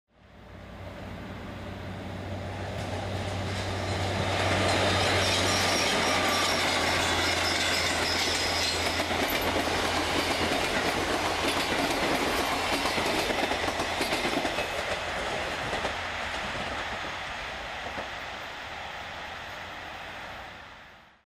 7 Augustus 2002 Eindhoven : NS 6502 Met een Virm op transport.
7 Augustus 2002 Eindhoven : NS 6502 Met een Virm op transport.